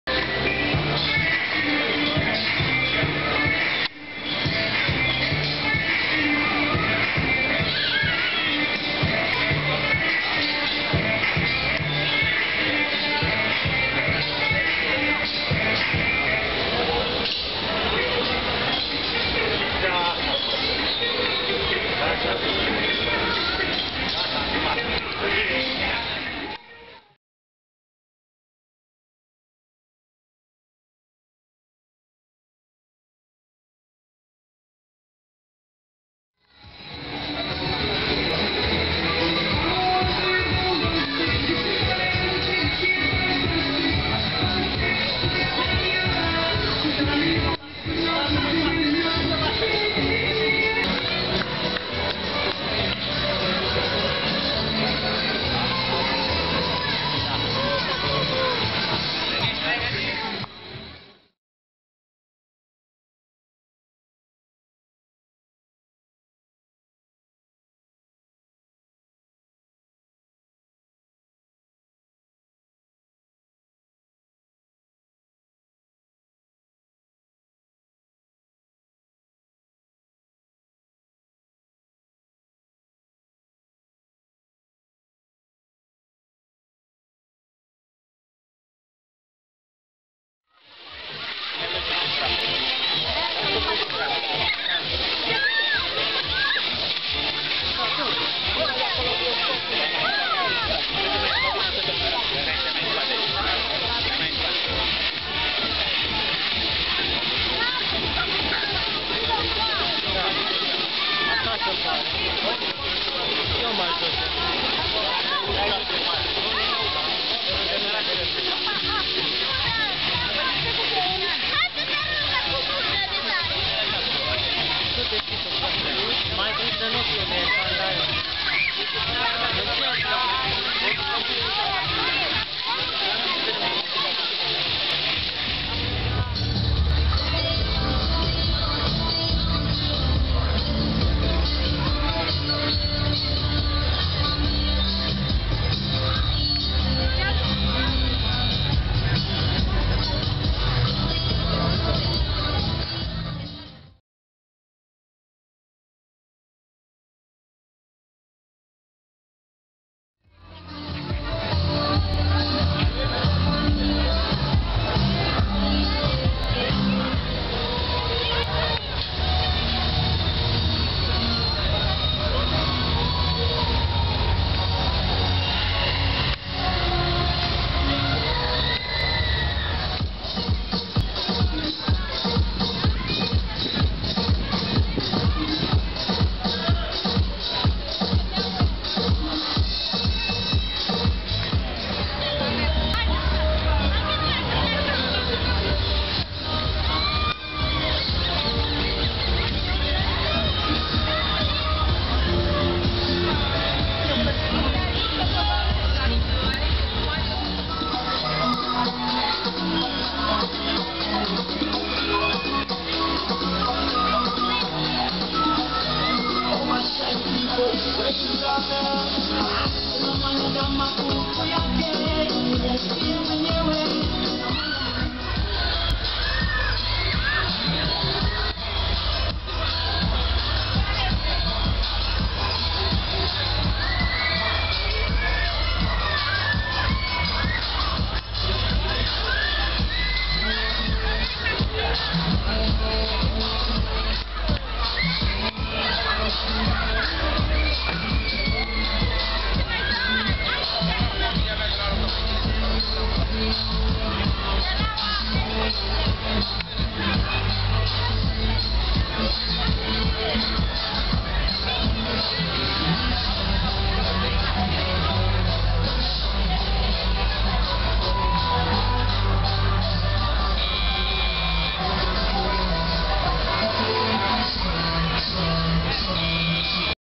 Duminica seara in Oraselul Copiilor, 26 iunie 2016
Seara la distractie si la plimbare prin parc.
 Sunday evening for some fun at the Children's Town amusement Park.